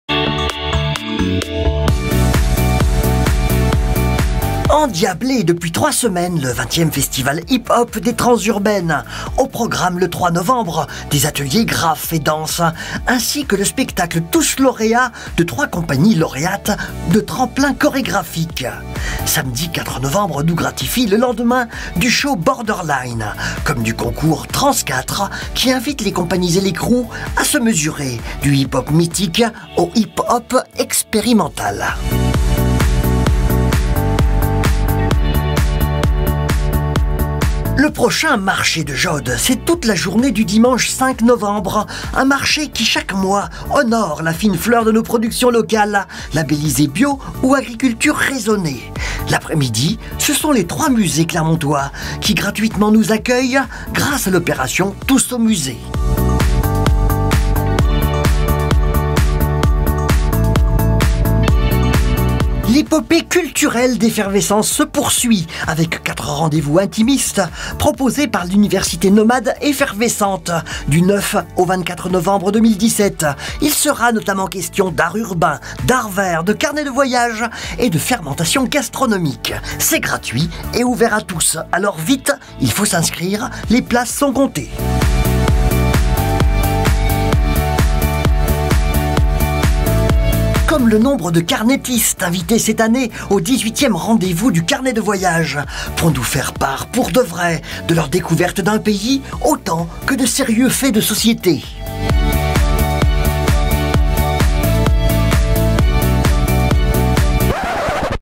En bref, semaine du 2 au 9 novembre 2017
Trans'urbaines 2017
C'est à un cocktail explosif de spectacles professionnels, events, battle, concerts que nous sommes conviés. Egalement tremplin pour la scène artistique locale, le festival met aussi ses artistes en devenir sur le devant de la scène.
Les Trans’urbaines, sont toujours l'opportunité d’aborder la culture hip hop de manière privilégiée. Des ateliers, invitations à danser, graffer, rapper, offrent à tous l’occasion de se perfectionner ou tout simplement de faire ses premiers pas.

Jaude fait son marché
C'est jour de marché à Clermont-Ferrand, dimanche 5 novembre, toute la journée de 9h à17h, à l'occasion du 40ème anniversaire du Parc Régional des Volcans d'Auvergne.  Des producteurs fermiers et des artisans d'art des Volcans d'Auvergne seront exceptionnellement présents pour nous présenter leur Marché Incontournable de l'Appétit et du fait-Main, le fameux MIAM.
Favoriser les circuits courts et notamment les producteurs locaux permet de préserver notre environnement. La Ville propose donc aux Clermontois, une fois par mois,  un marché favorisant les productions locales et bio. Ce marché se tient  Place de Jaude, un lieu emblématique de la ville.

Tous aux musées
Entrées gratuites, chaque premier dimanche du mois, dans les trois musées nationaux clermontois. Sont donc concernés le Musée d'Art Roger Quilliot, le Musée Bargoin d'Archéologie et d'Arts Textiles ainsi que le musée Lecoq d'Histoire Naturelle.

Université nomade effervescente
L' aventure continue, Effervescences vous propose des rendez-vous à géométrie variable en complicité avec de nombreux acteurs artistiques, scientifiques et technique. Ainsi s'explorent de nouveaux possibles au coeur de rendez-vous et d'événements phares de la vie clermontoise.
Rendez-vous de novembre 2017 :
- 9 novembre : Histoires de fermentation.
-15 novembre : Arts verts.
-19 novembre : Retour de promenade.
-24 novembre : Embrasser la rue.

18e Rendez-vous du Carnet de Voyage.
Le 18ème Rendez-vous du Carnet de Voyage les 17, 18 et 19 novembre 2017,  se donne pour objectif de promouvoir le carnet et le récit de voyage.
C'est tous les ans au mois de novembre que Clermont-Ferrand devient le temps d'un week-end le carrefour de rencontres entre les carnettistes  amateurs et professionnels et le grand public.
De l'aquarelle à la bande dessinée, du carnet traditionnel aux carnets numériques et sonores, tous les styles sont représentés, faisant du Rendez-vous clermontois la plus grande manifestation du genre et un moment unique de découverte et de partage.